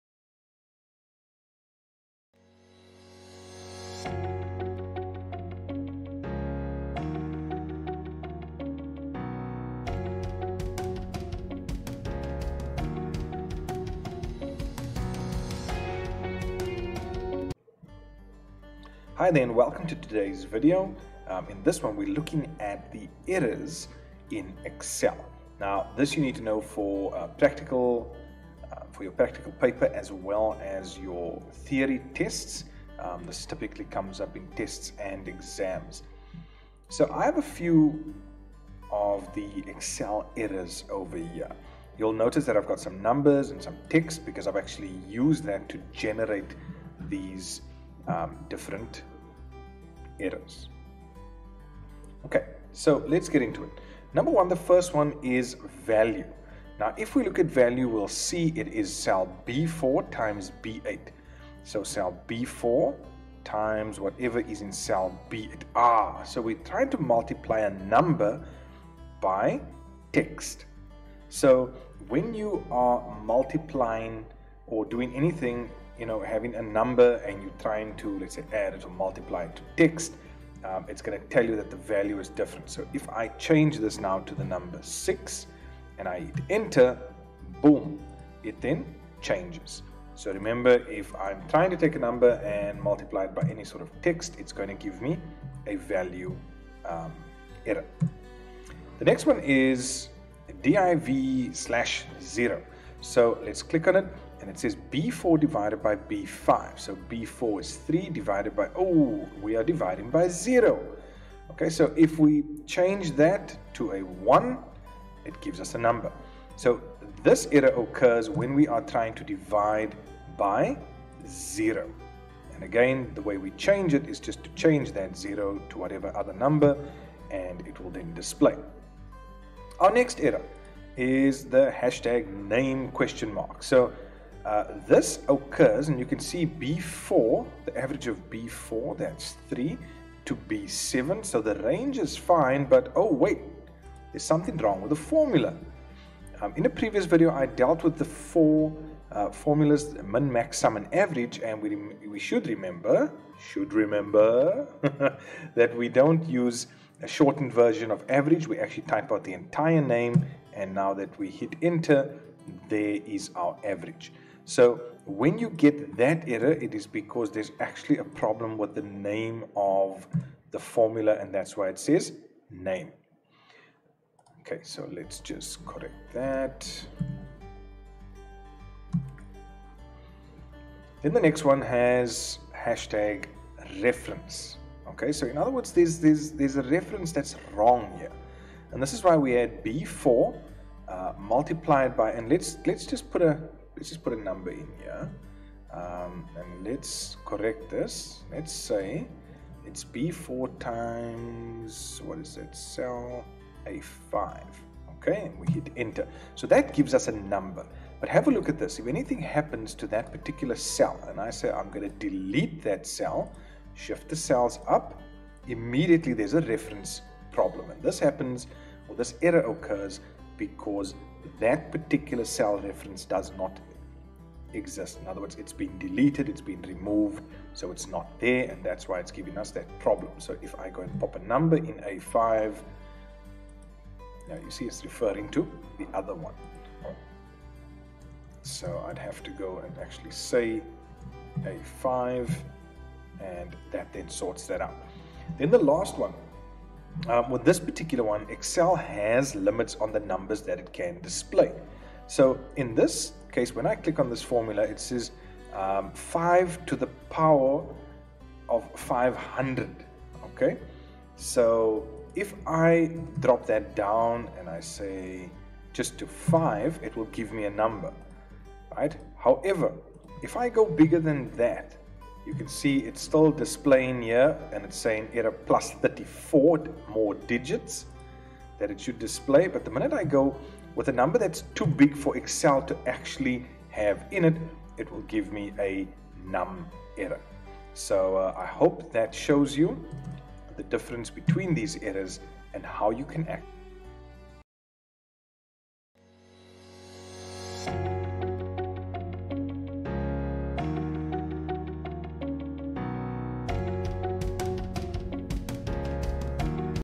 Errors in Excel
If you're having problems with errors in Excel, then check out this video for more help. In this video, we'll show you how to solve common Excel errors and problems.

Whether you're a beginner or a power user, this video will help you solve common Excel errors and problems. We'll also provide some helpful tips on how to use Excel effectively. So if you're struggling with errors in Excel, check out this video for help!